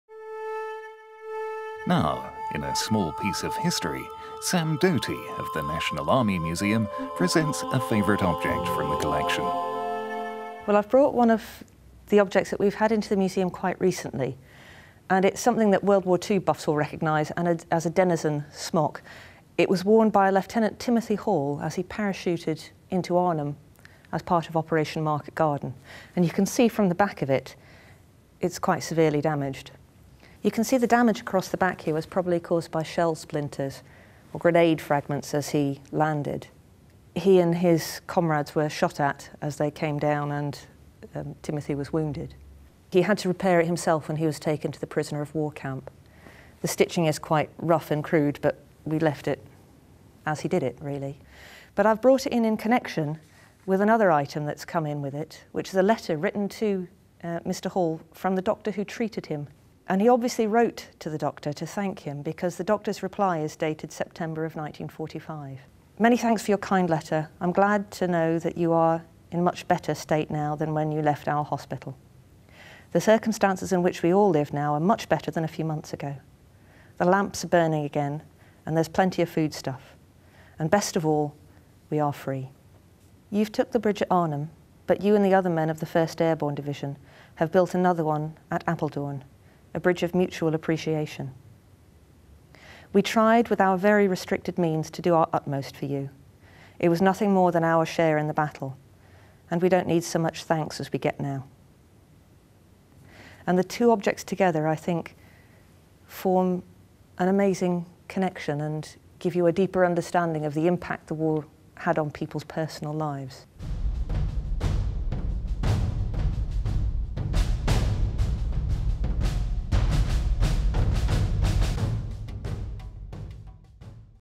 Denison Smock
Sam Doty of the National Army Museum presents a favourite object from the Collection - a denison smock worn by a paratrooper during Operation Market Garden (September 1944).